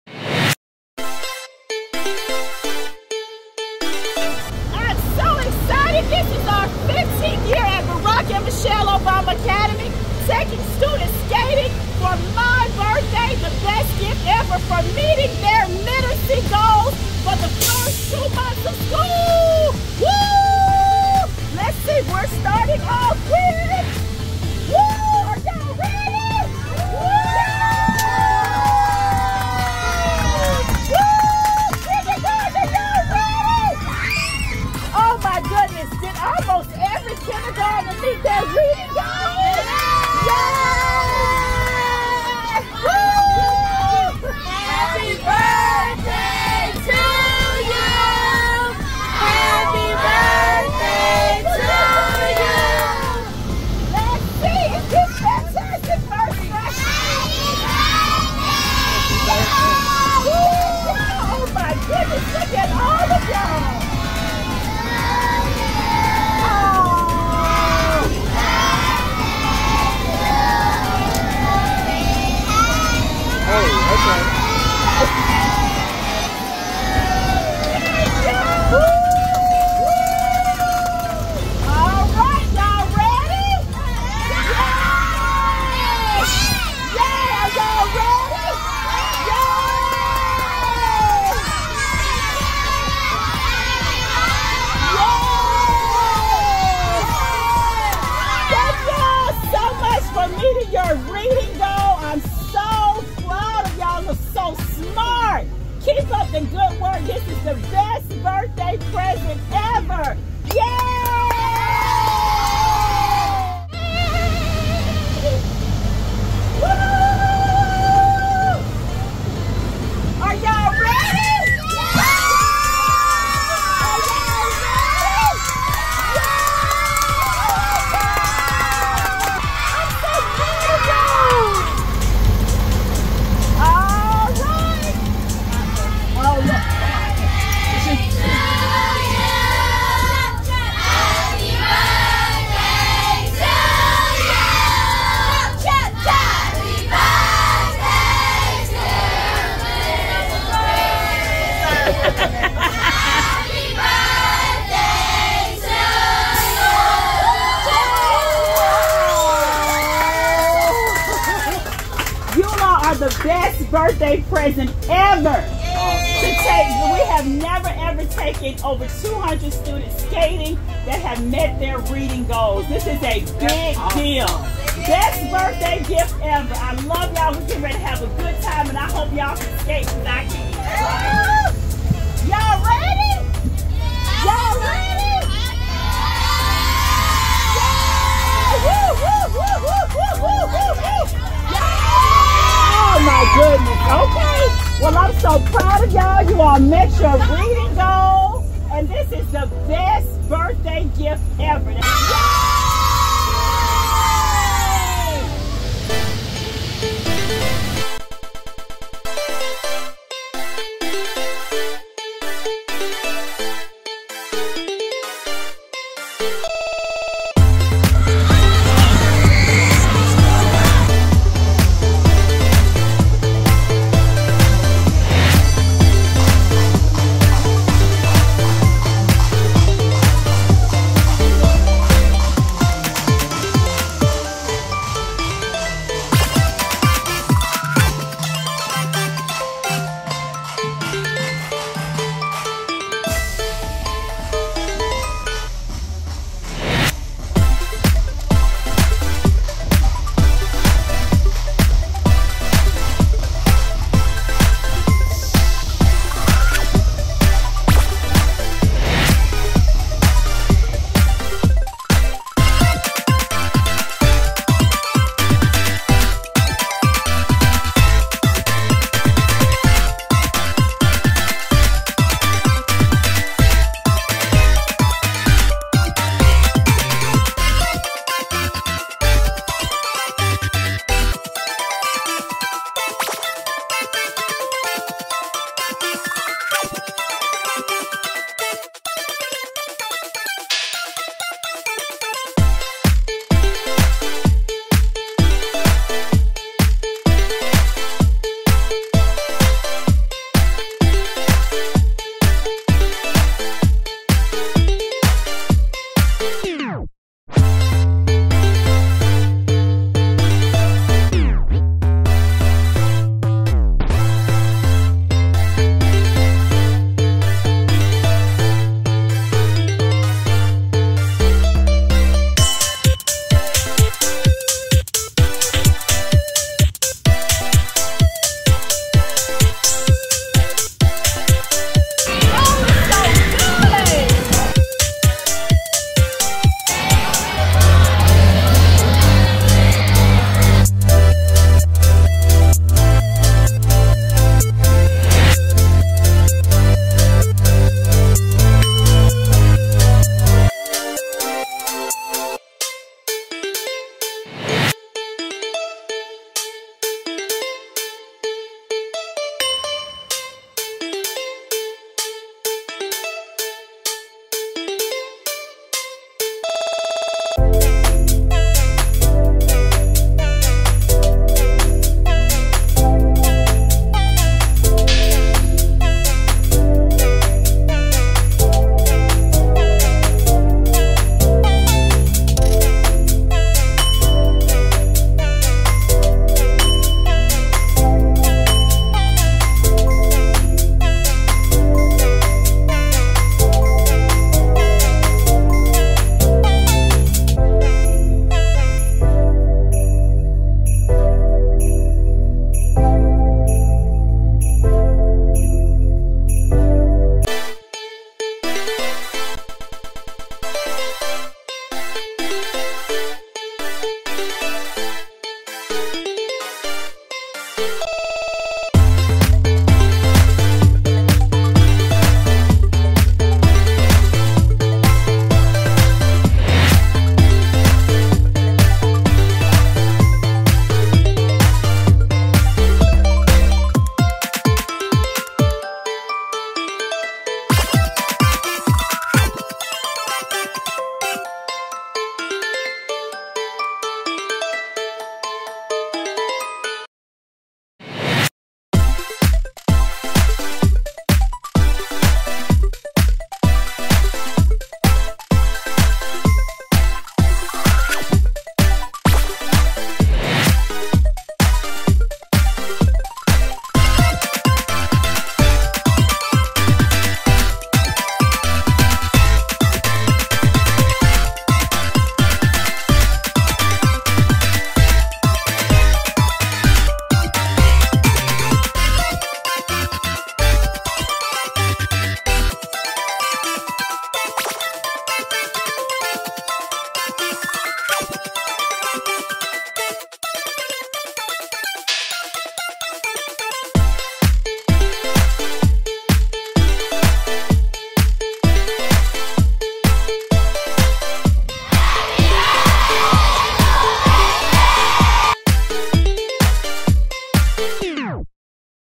BAMOEAGLES Celebrates Principal Christians Birthday
BAMOEAGLES Celebrates Principal Christians Birthday with our annual Accelerator Reading(AR) skate party! Over 200 students met or exceeded their AR goals!